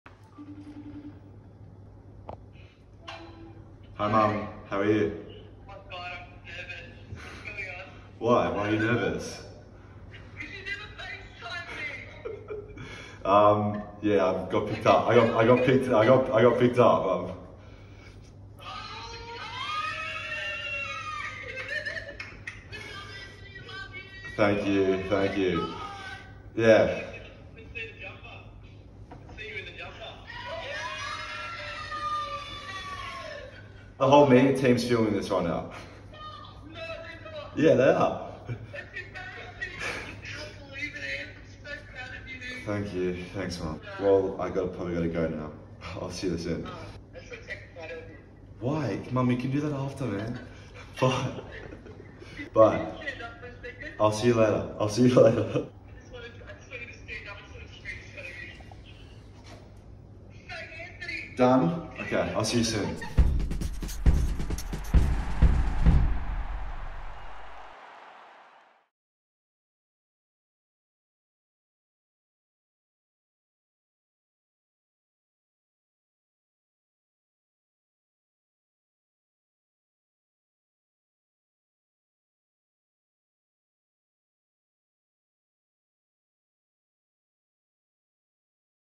'Oh my god!' Caminiti family reacts to special news
Anthony Caminiti calls his mum to deliver the news that he's officially a Saint.